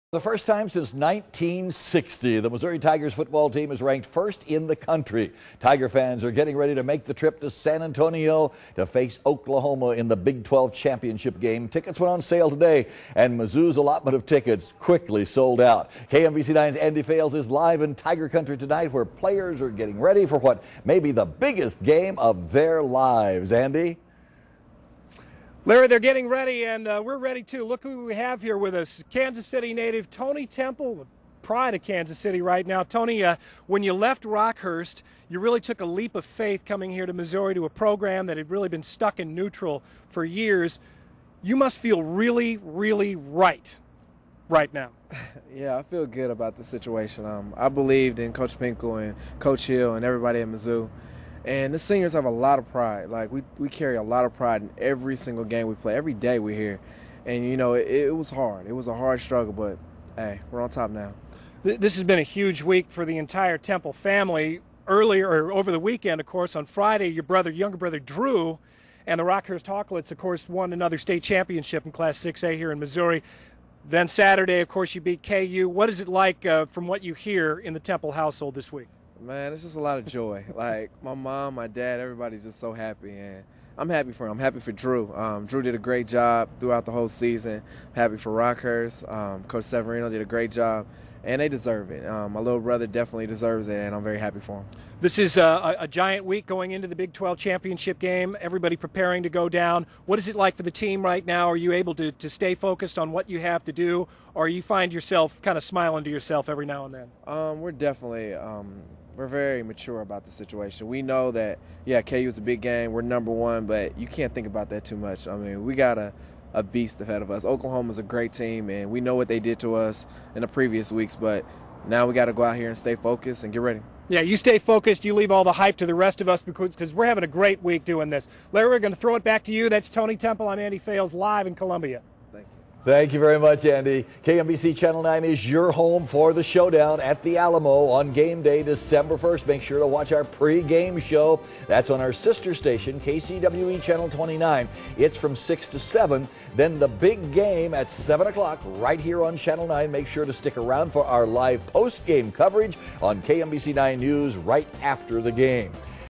Tony Temple: It's Been Hard Struggle For MU
KMBC's Andy Fales talks with MU running back Tony Temple.